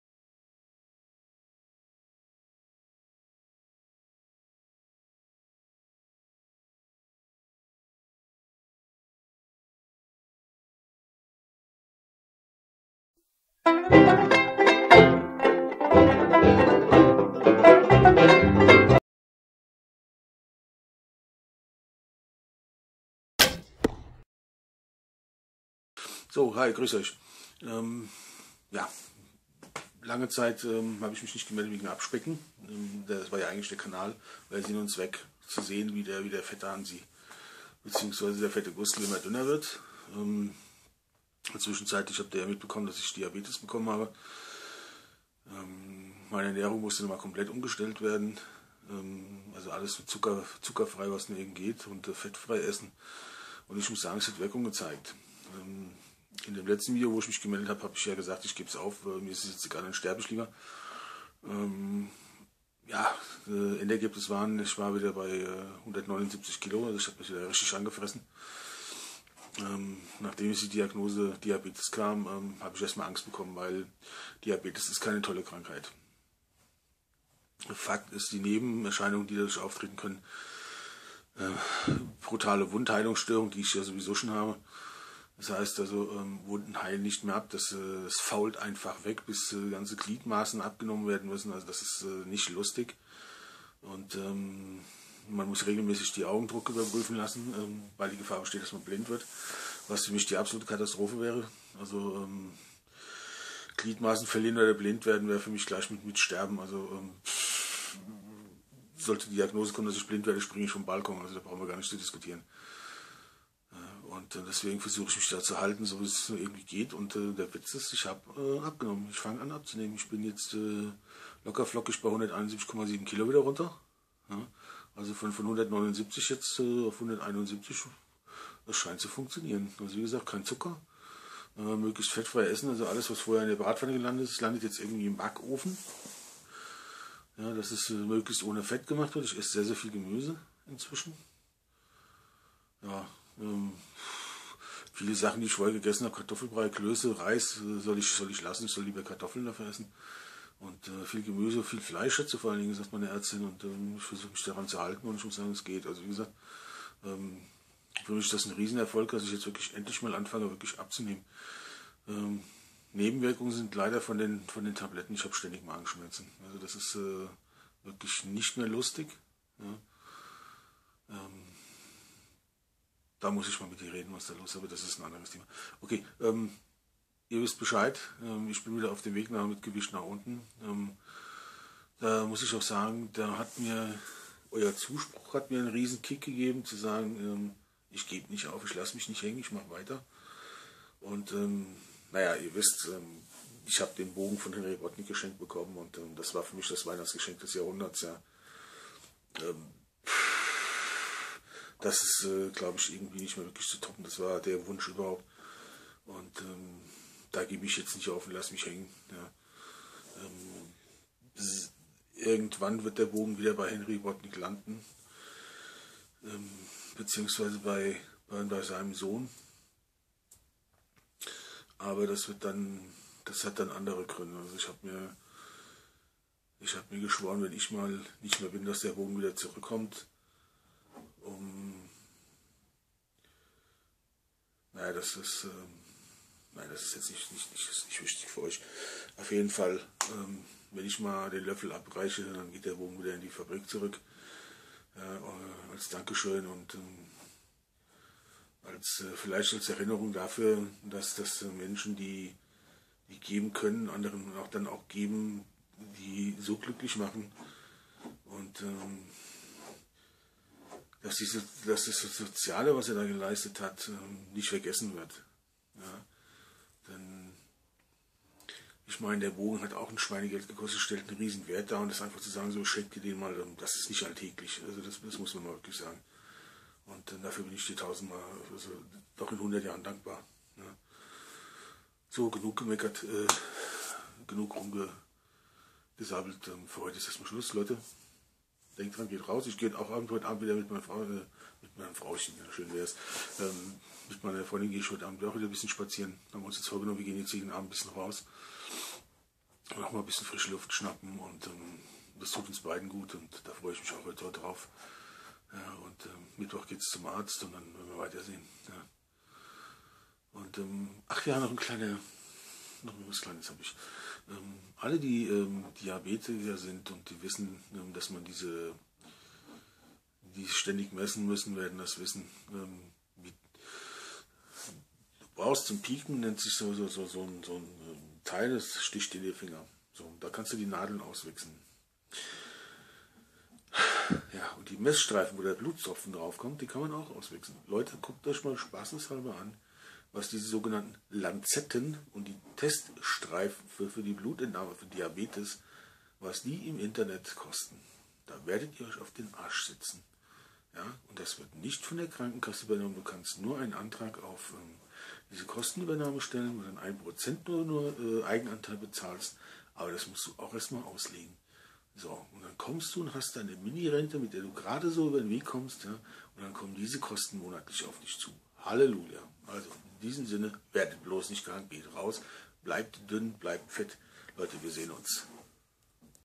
abspeck news februar 2017
Die Diabetis zeigt erste Wirkungen...

Für mich beste Bögen der Welt: Henry Bodnik´s Bearpaw

Vielen dank.